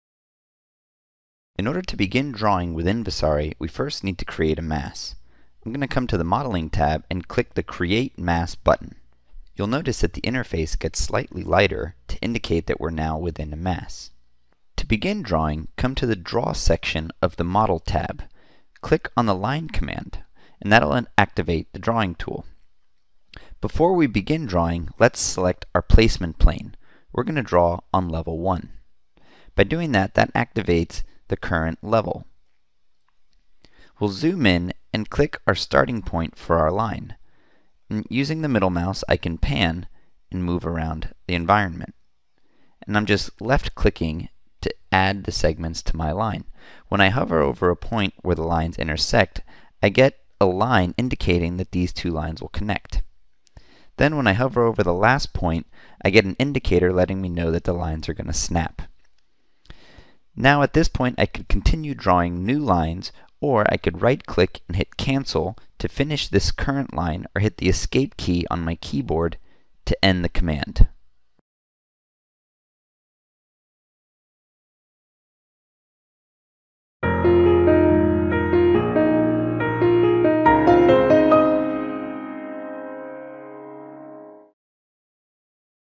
Vasari Essential Skills - Draw
How to draw lines and curves in Project Vasari